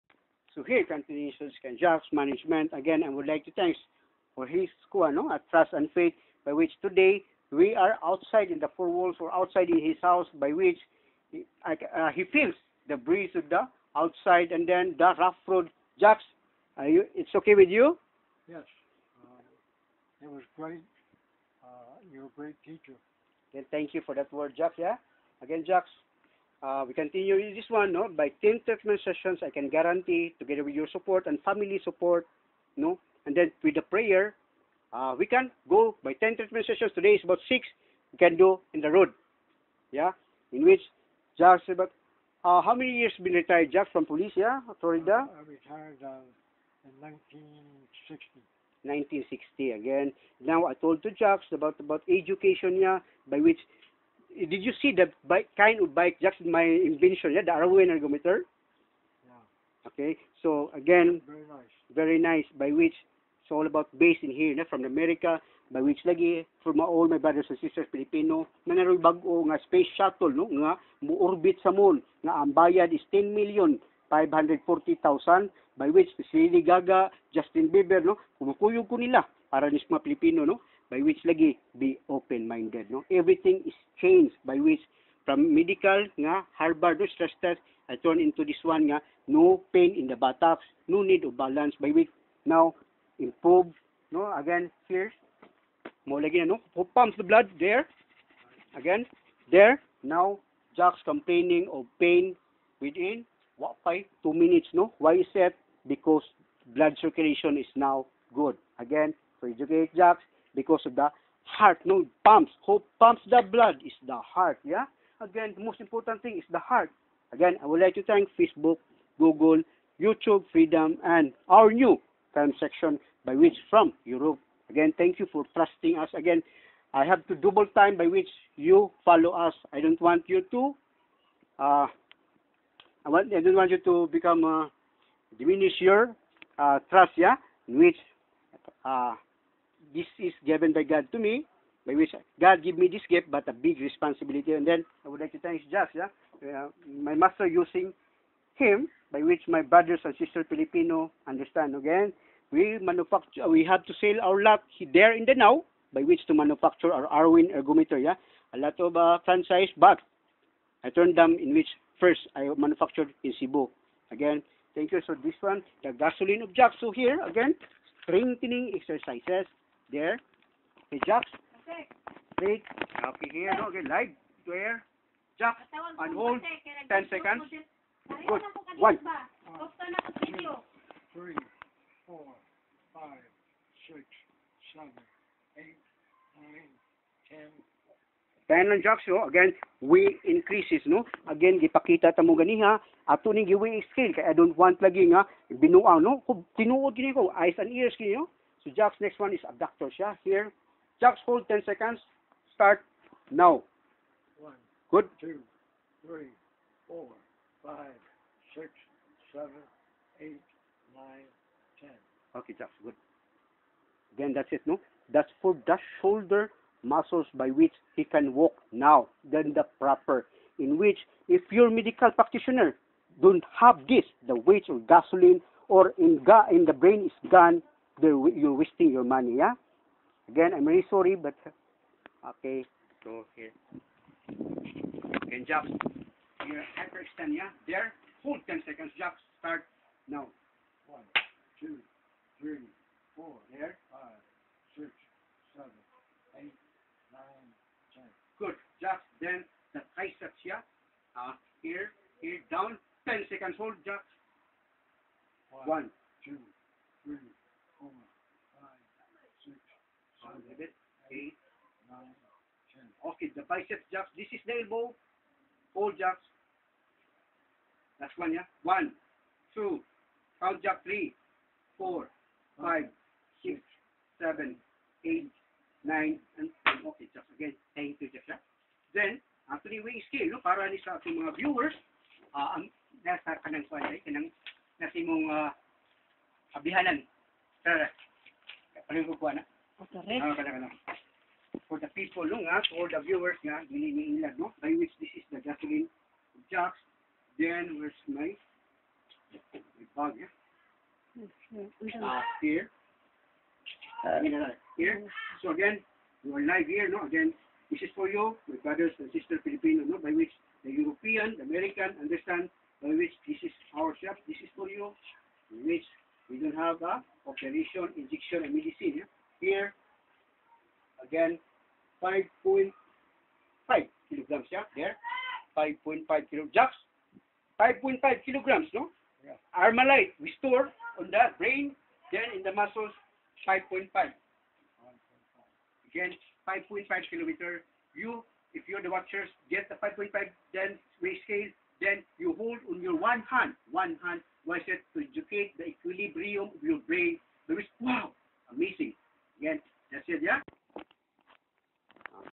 STRENGTHENING / Gasoline in Muscles and in Brain ARMALITE GUN
Harthys Pure Care.. Episode / Treatment NO. 6 by which can Walk his Own by which her Wife told ne that from Bed to The Comfort Room.. why/ OPEN MINDID Lesten to what he SAI COz there COUNTRY AMERICA SUPHISTICATED Push Botton rata we Are all Dead.. but Why ?